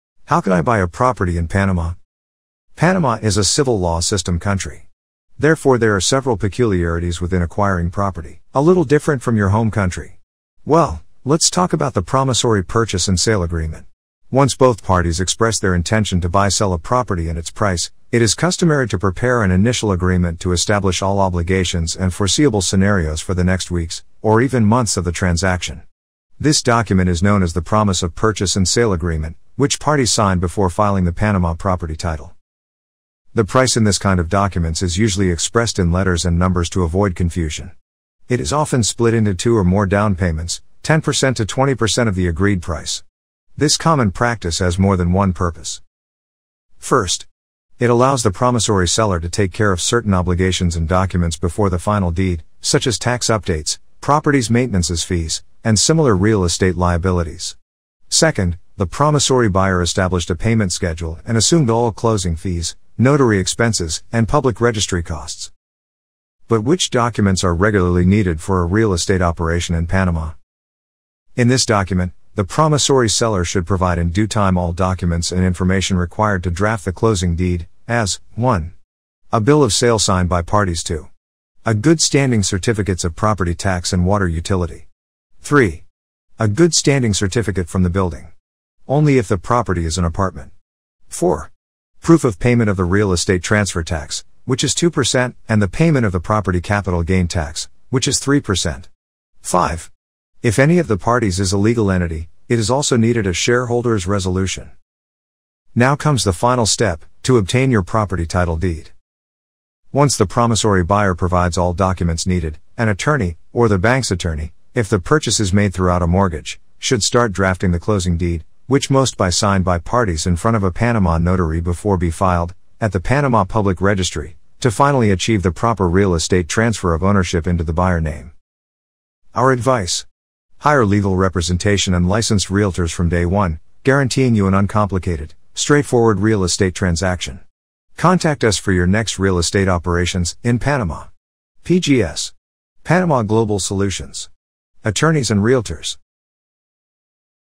►► How to Buy Real Estate in Panama
How can I Buy a Property In Panama?

Panama is a Civil Law system country, therefore there are several peculiarities within acquiring property, a little different from your home country.

Let´s talk about first the "Promissory Buy-Sell Agreement."

Once both parties express their intention to buy-sell a property and its price, it is customary to prepare an initial agreement to establish all obligations and foreseeable scenarios for the next weeks (or even months of the transaction). This document is known as the "Promise of Purchase and Sale Agreement," which parties sign before filing the Panama property title.

The price in this kind of documents is usually expressed in letters and numbers to avoid confusion. The property price is often split into two or more down payments, 10% to 20% of the agreed price. This common practice has more than one purpose.

First, it allows the Promissory Seller to take care of certain obligations and documents before the final deed, such as tax updates, properties Maintenances fees, and similar real estate liabilities. Second, the promissory buyer established a payment schedule and assumed all closing fees, notary expenses, and public registry costs.

But which documents are regularly needed for a real estate operation in Panama?

In this document, the promissory seller should provide in due time all documents and information required to draft the closing deed, such as:

1. A Bill of Sale signed by parties
2. A good standing certificates of property tax and water utility.
3. A good standing certificate from the building. Only if the property is an apartment.
4. Proof of payment of the real estate transfer tax (2%) and the property capital gain tax (3%).
5. If any of the parties is a legal entity, a Shareholders' Resolution is allowing the operation.

Now comes the final step to obtain your property title deed.

Once the promissory buyer provides all documents needed, an attorney (or the bank's attorney, if the purchase is made throughout a mortgage) should start drafting on the closing deed, which most by signed by parties in front of a Panama notary before be filed in the Panama Public Registry, to finally transfer the ownership of the real estate into the buyer name.

Our advice. Hire legal representation from day ONE, guaranteeing you an uncomplicated, straightforward real estate transaction.

►► PGS Attorneys and PGS Realty.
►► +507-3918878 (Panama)

created by Edmond Dantès, Kelly Lacy en Pexels, Lisandro G Aguilar, Pavel Danilyuk, Cottonbro, and Pressmaster.